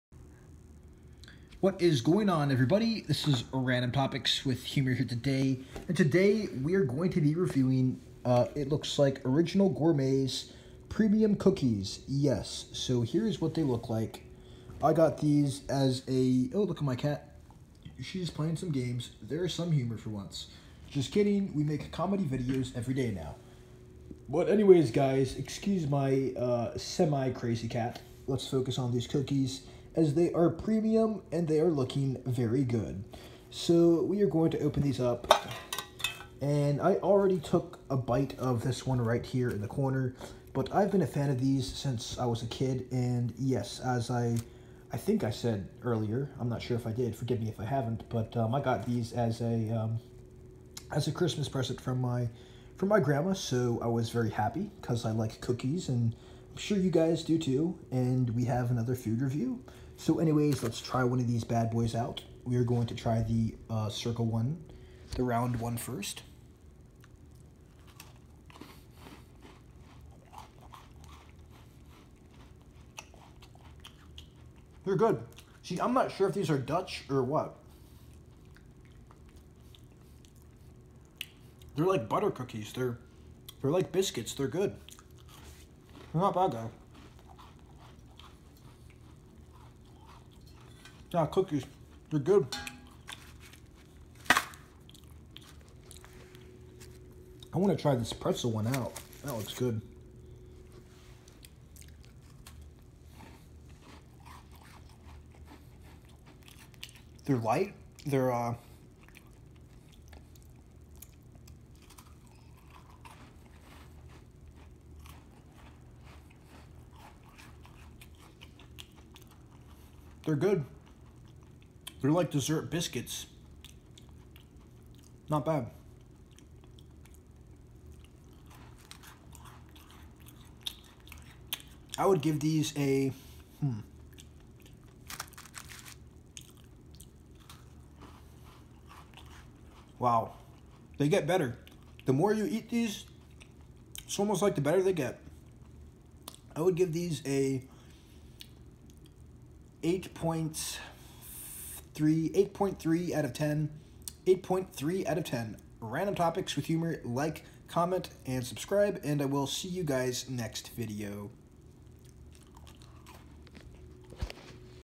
Original Gourmet Premium Cookies Review
In this video we will be reviewing original gourmet's premium cookies.